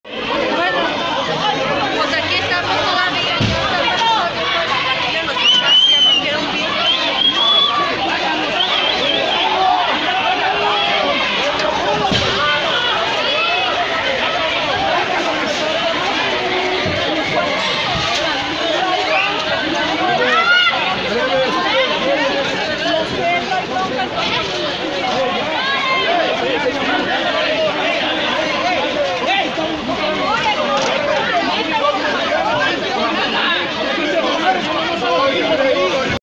Vive mañana de terror primaria en Insurgentes
Conductor de vehículo intencionalmente impactó su auto contra el portón de la escuela y amenazó a maestros